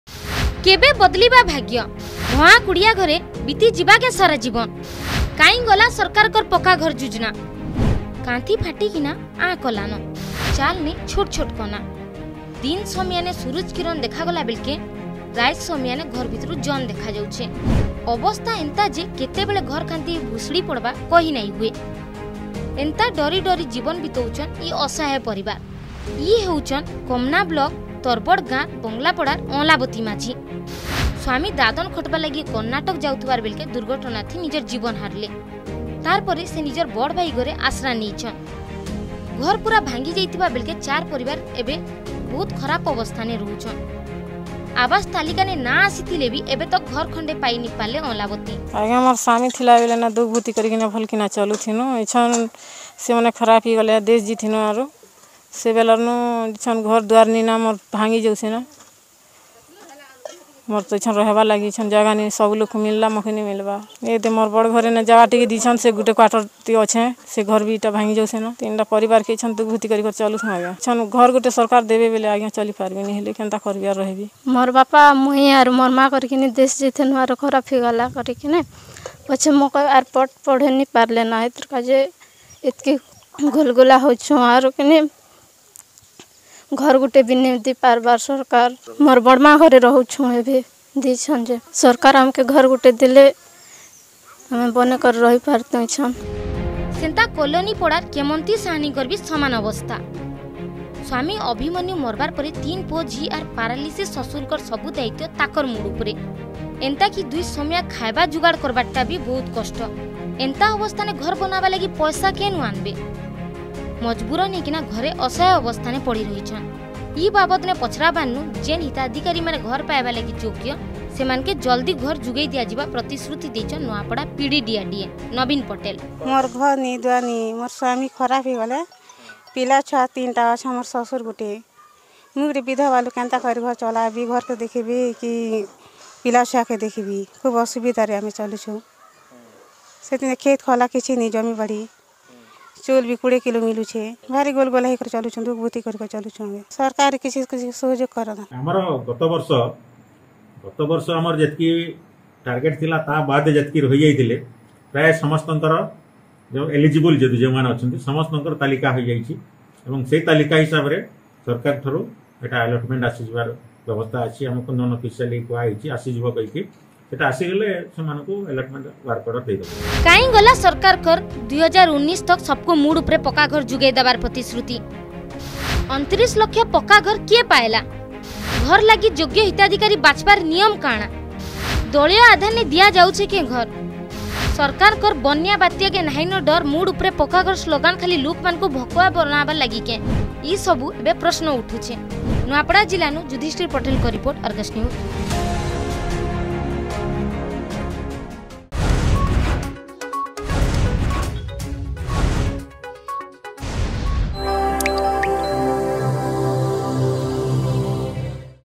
Awas Yojana: କେବେ ବଦଲିବା ଭାଗ୍ୟ ?
ସରକାର କହୁଛନ୍ ବିକାଶ୍ ହଉଛେ। ରାଏଜନ ଗରିବ ମାନକେ ମୈଲିକ୍ ସୁବିଧା ଦିଆଯାଉଛେ। ହେଲେ ଅସଲ୍ ରେ ଇ ଛବି ଟିକେ ଅଲ୍ ଗା । ଏଭେ ତକ ଗରିବ ଦୁଖିଆରି ମାନକର ଲାଗି ଯୁଜନା ପାଲଟିଛେ ସାତ ସପନ୍